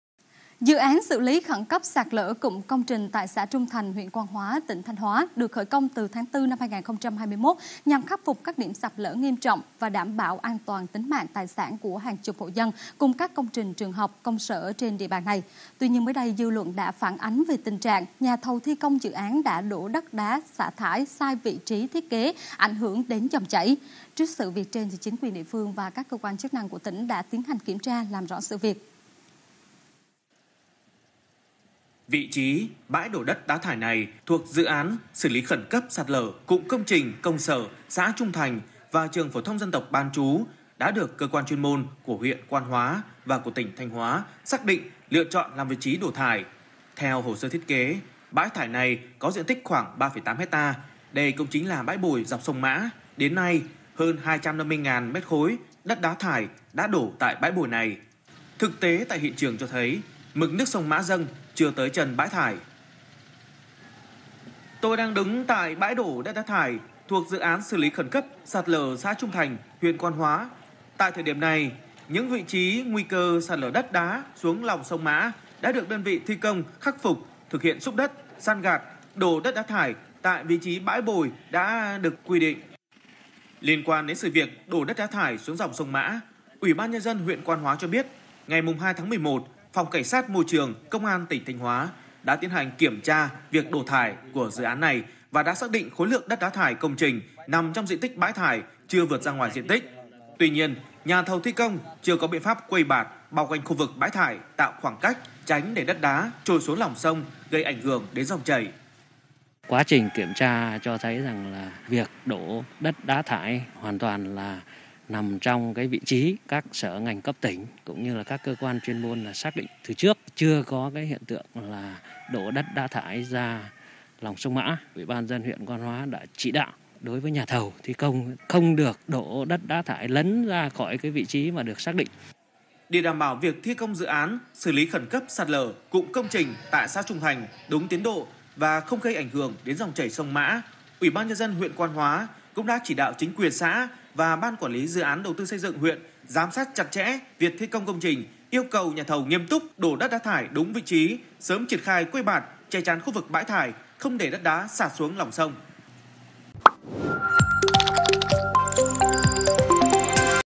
vtv8- Thanh Hoá: Làm rõ việc nhà thầu thi công đổ đất đá thải xuống sông Mã
Dự án xử lý khẩn cấp sạt lở cụm công trình tại xã Trung Thành, huyện Quan Hoá, tỉnh Thanh Hoá được khởi công từ tháng 4/2021 nhằm khắc phục các  điểm sạt lở nghiêm trọng nhằm đảm bảo an toàn tính mạng , tài sản của hàng chục hộ dân và các công trình trường học, công sở trên địa bàn này. Tuy nhiên mới đây, dư luận đã phản ánh về tình trạng nhà thầu thi công dự án đã đổ đất đá thải sai vị trí thiết kế, ảnh hưởng đến dòng chảy. Trước sự việc trên, chính quyền địa phương và các cơ quan chức năng của tỉnh đã tiến hành kiểm tra, làm rõ  sự việc. (NCV8 , 21/11/2021)